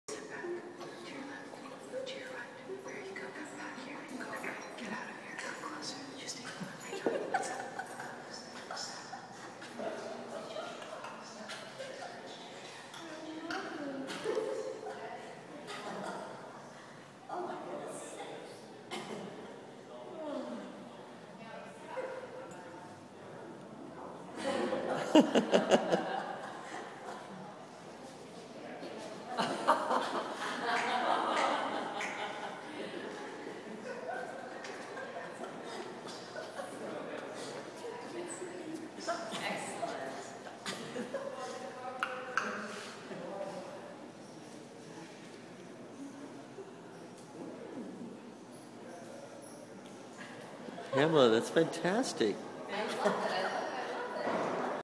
Vera Lundahl Dancing at Pamela Zs
This was an installation by Pamela called Ottimista Pessimista